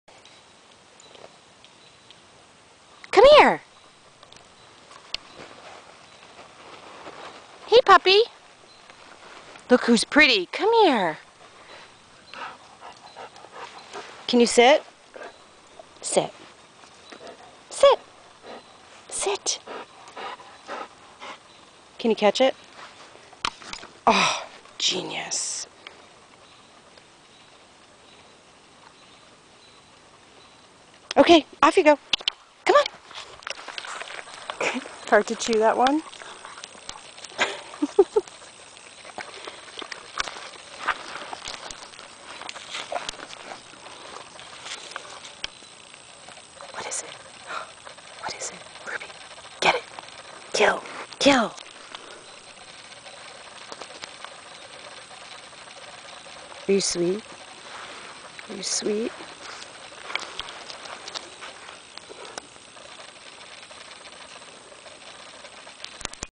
Catch!
Genius hound. Took forever to learn to catch.